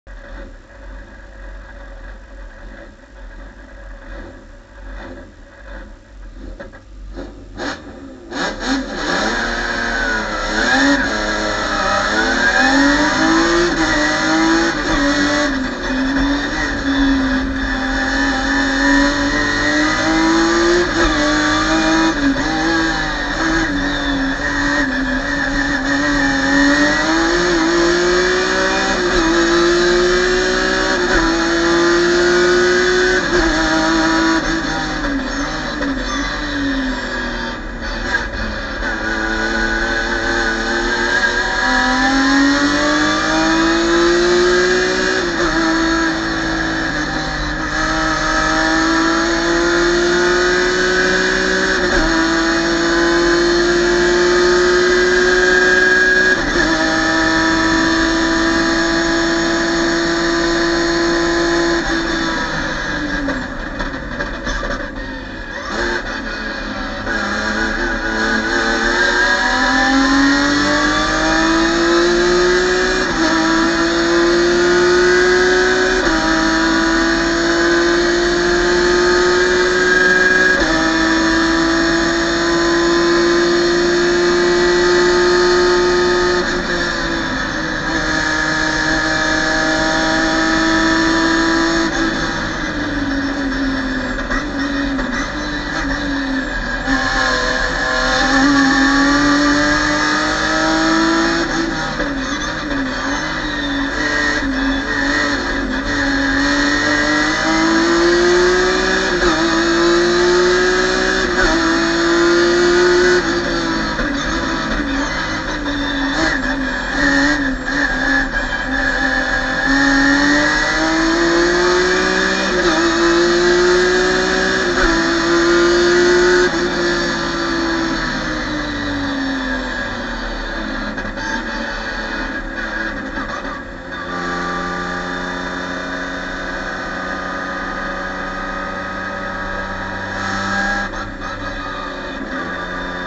Pavel Kotek BMW M3 GMS Albrechtický kopec 2.-3.8.2019
Po dlouhých 7 letech se uskutečnily závody na kopci "Laudon". Náš onboard je z nedelní druhé závodní jízdy, kdy jsme zajeli nejlepší čas víkendu 1.55.93.